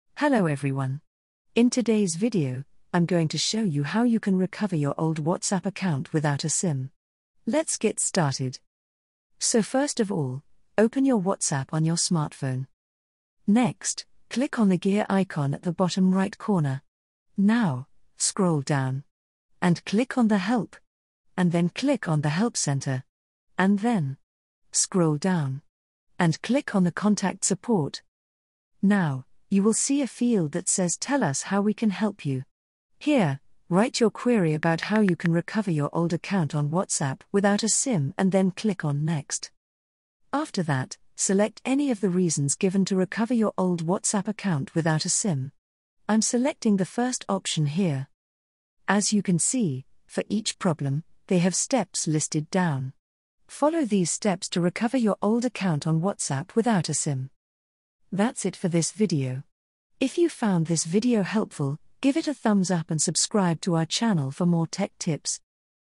How To Recover Old WhatsApp Account Without Sim | Easy Guide
Learn how to recover your old WhatsApp account without a SIM card. This step-by-step tutorial will guide you through the process of regaining access to your WhatsApp account even if you no longer have your original phone number. Follow these tips and tricks to ensure you can reconnect with your contacts and restore your chat history.